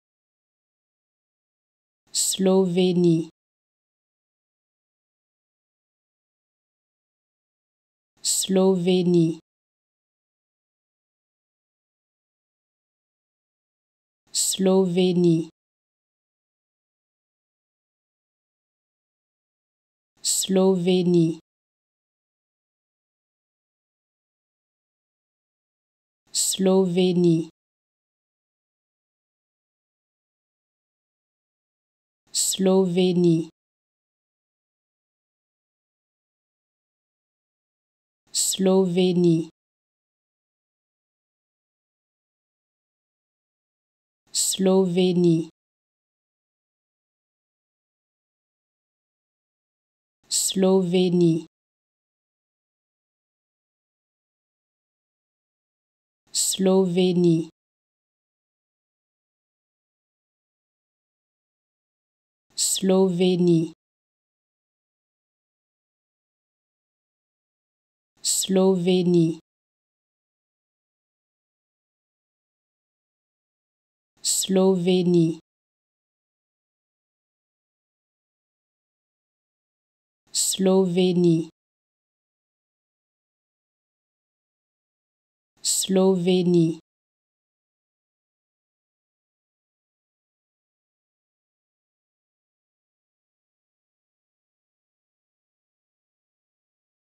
How to Pronounce ''Slovénie'' (Slovenia) Correctly in French
Learn how to say and properly pronounce ''Slovénie'' in French with this free pronunciation tutorial.